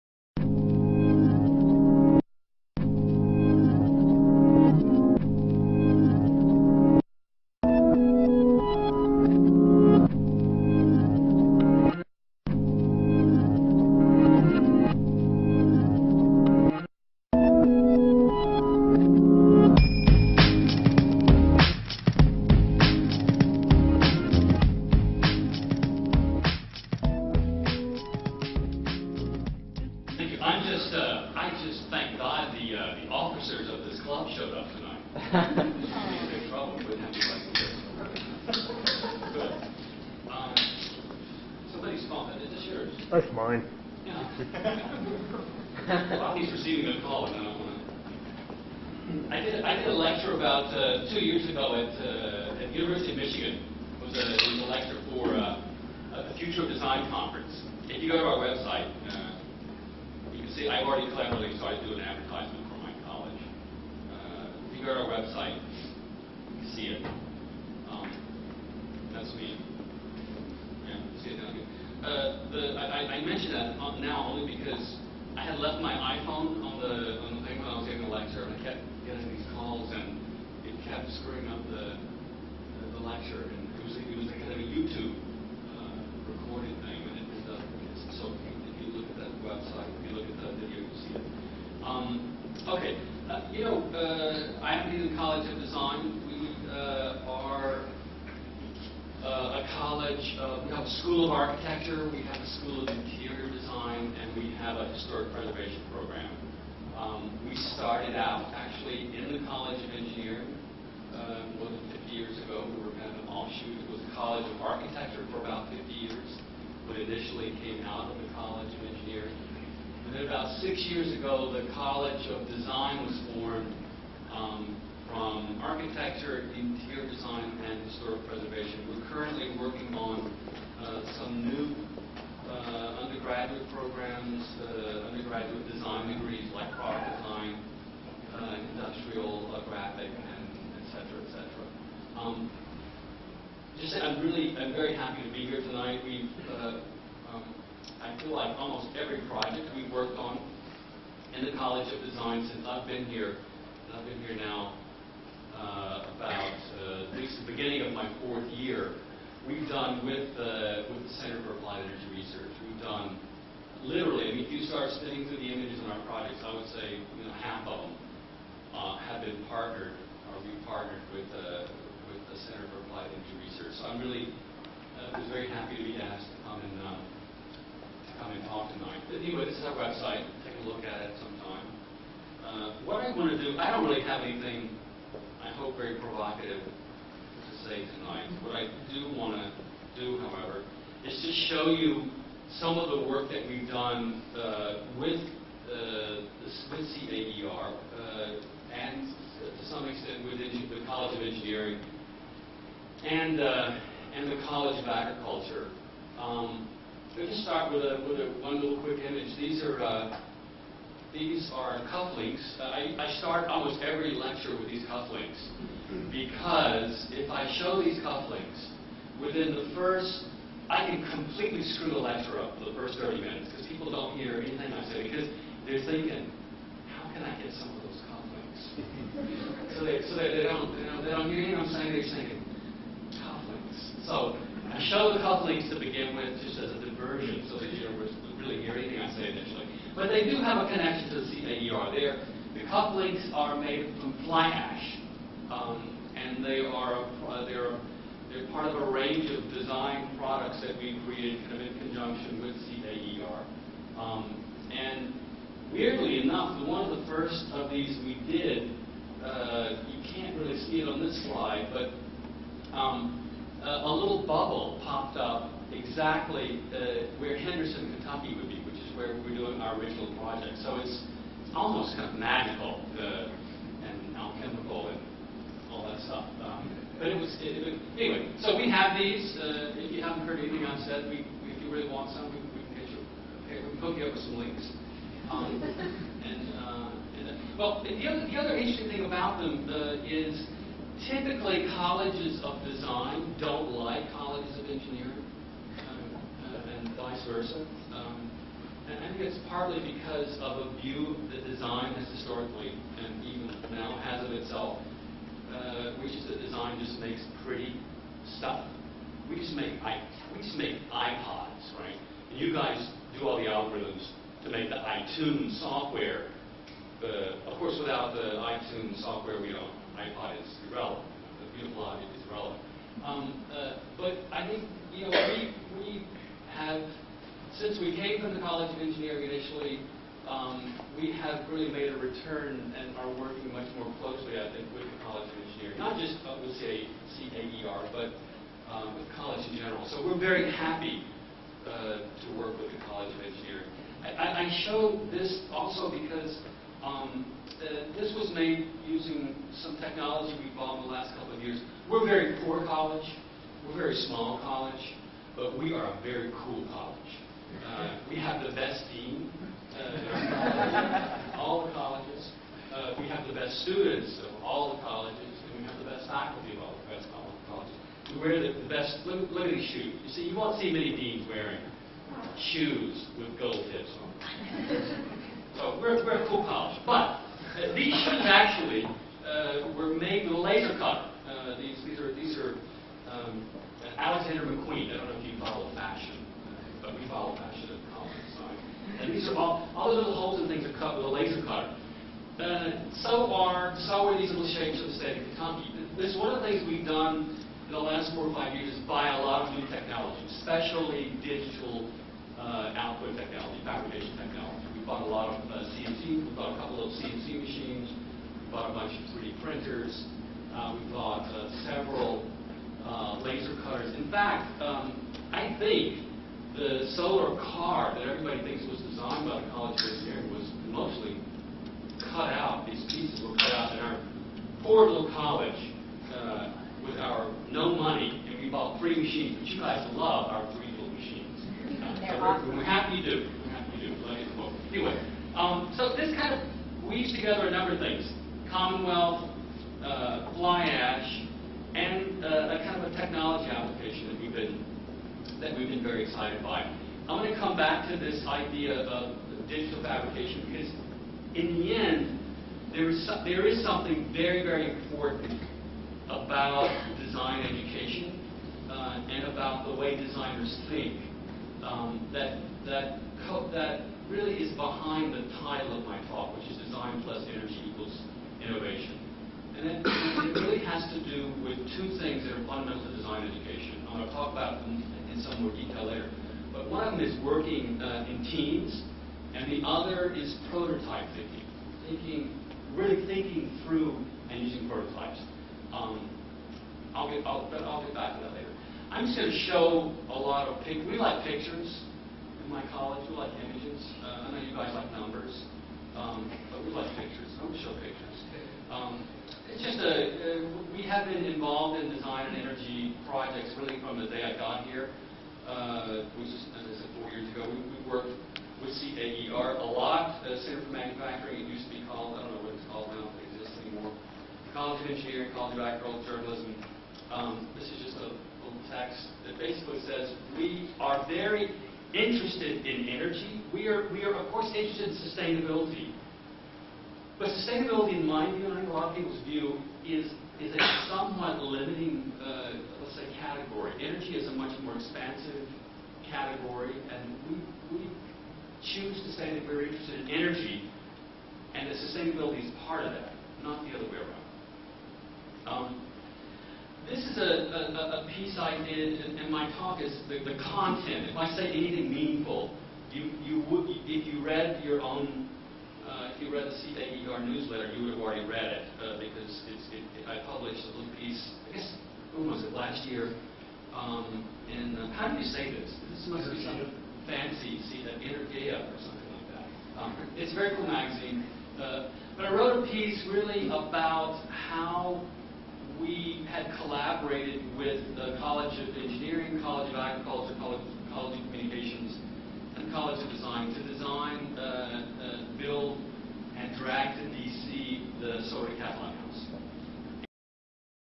Energy and Design 1/6
Lecture by the Dean of the Uky Colllege of Design.  Dr. Speaks.